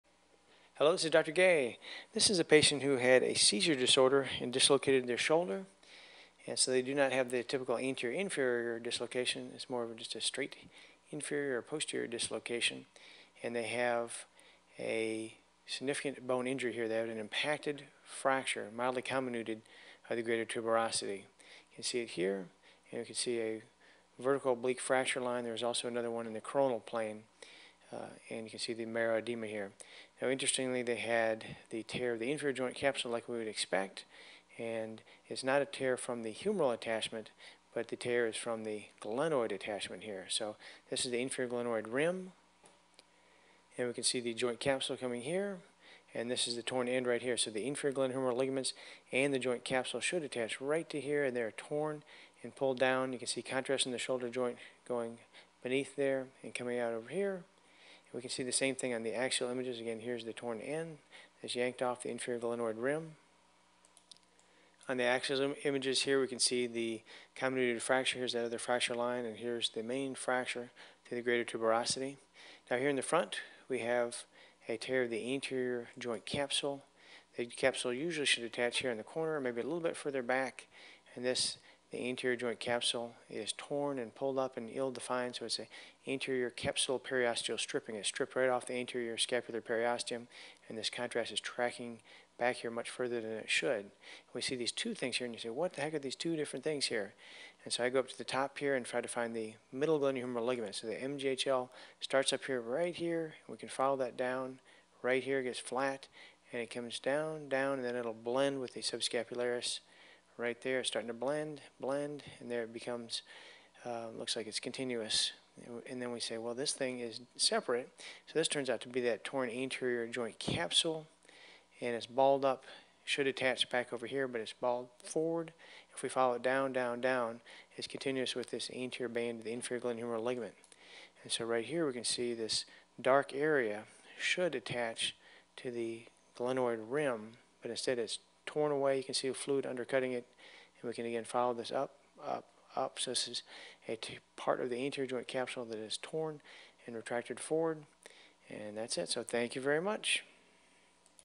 Shoulder MRI Scan - Inferior Shoulder Dislocation
How to read and identify Inferior Shoulder Dislocation in a Shoulder mri scan.

An inferior shoulder dislocation is the least common form of shoulder dislocation. The condition is also called luxatio erecta because the arm appears to be permanently held upward, in fixed abduction. The patient will often present with their hand placed on the head or near it.

Dr. Brian Gay from First Look MRI explains how to read a Shoulder MRI scan.